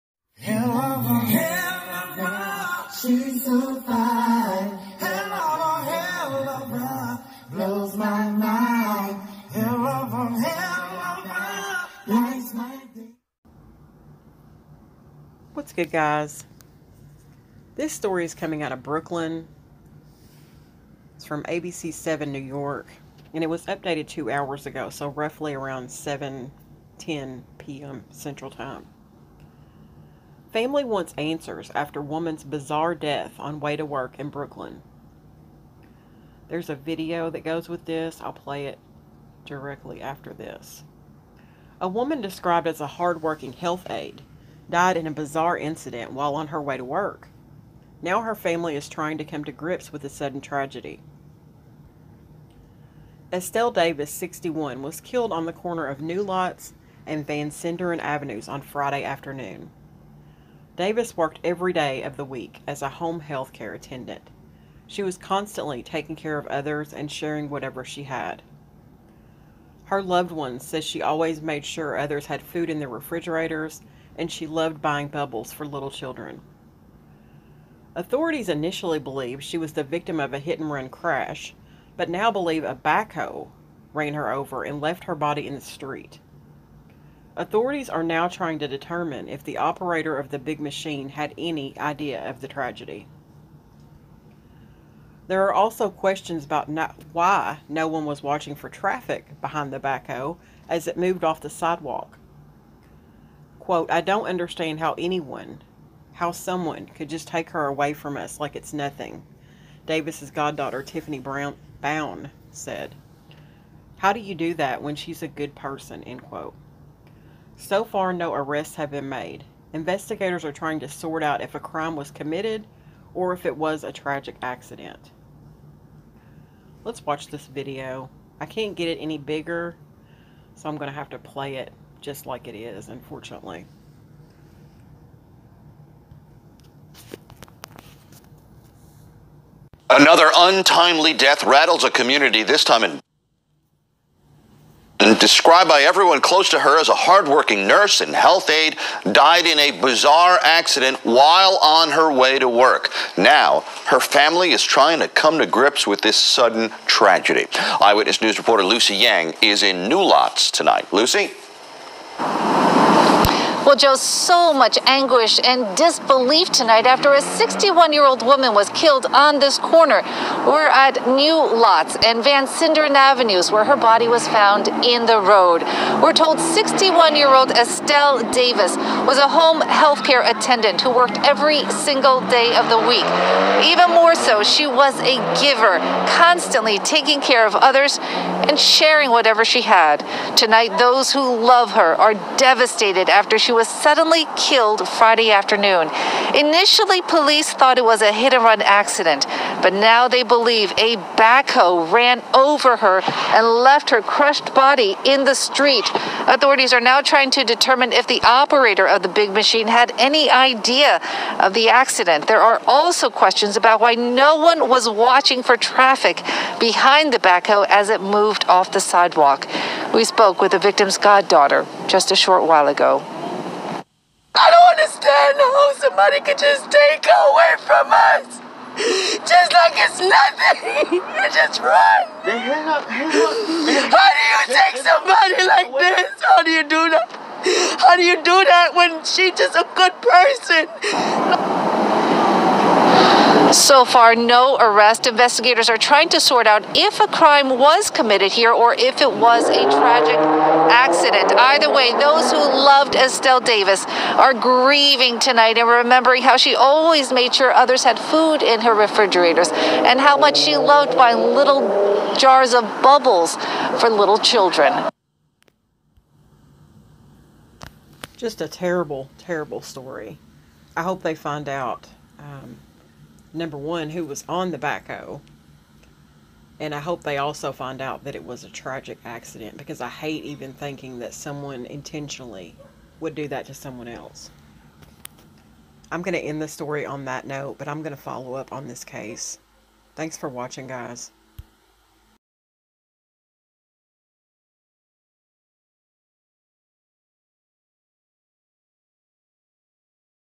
Woman's Bizarre Death on Way to Work
This video is about 61-year old Estelle Davis, a healthcare worker, who was ran over by a backhoe on her way to work on Friday in Brooklyn.  Police are unsure whether the backhoe operator even knew what happened.